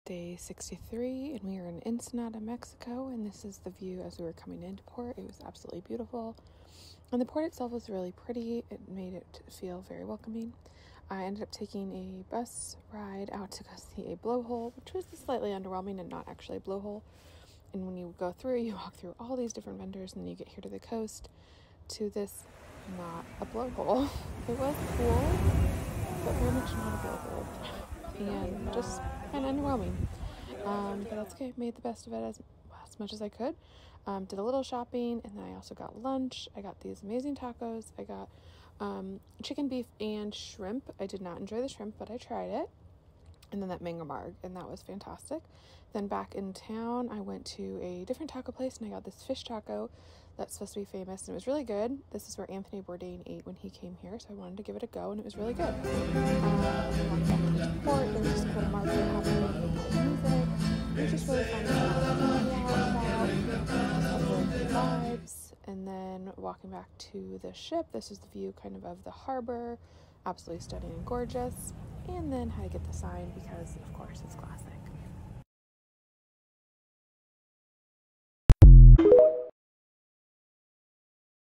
Day 63; Ensenada, Mexico (2/10/ 24) on the Ultimate World Cruise
Day 63; Ensenada, Mexico (2/10/ 24)
We had a beautiful sail coming into port and the town looked inviting and I was ready to set off. I made my way to what I was told was one of the three top blowholes in the world. But it was not a blowhole and pretty underwhelming in my opinion. But I did a little shopping and had a great lunch. I got a beef, chicken, and shrimp taco. Baja is known for its shrimp and fish tacos so I wanted to try them, I did not love the shrimp one, but I did love my mango marg!
Back in town, I walked around a bit and went to a taco stall that Anthony Bourdain ate at when he was here. I had a fish taco, and again, I don’t love seafood but wanted to try and it was really good. I then walked back to the ship and took in some of the fun live music happening at a community market.